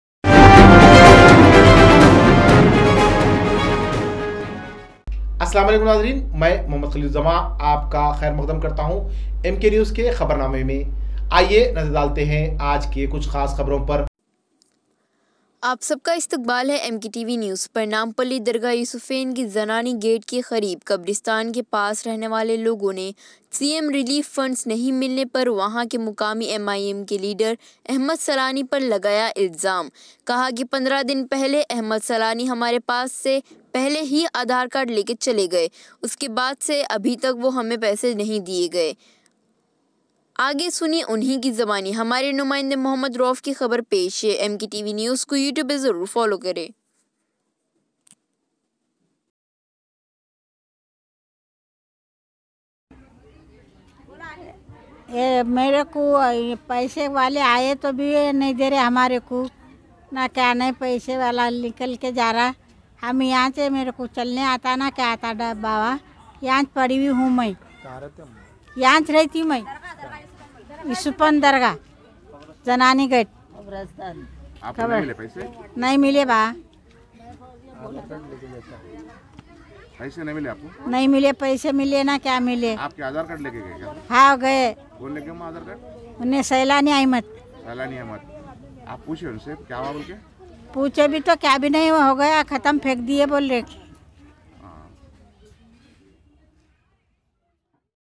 Nampally Dargah Yousufain Kay Zanani Gate Kay Kareeb Khabristan Kay Pas Rehne Wale Logou Ne Cm
Nampally Dargah Yousufain Kay  Zanani Gate Kay  Kareeb Khabristan Kay Pas  Rehne Wale  Logou Ne Cm Relief Funds Nahi Milne Per Waha Kay Muqaami MIM Kay Leader Ahmed Sailni Per Lagaya Ilzaam Kaha Kay 15 Din Pahele Ahmed Sailni  Hamare Pas Se Pahele Hi Adhar Card Lekar Gaye The..

Uskay Baad Se Abhi Tak Wo Hame Paise Nahi Diya Gaya..

Agay Suniye Unhi Ki Zubani..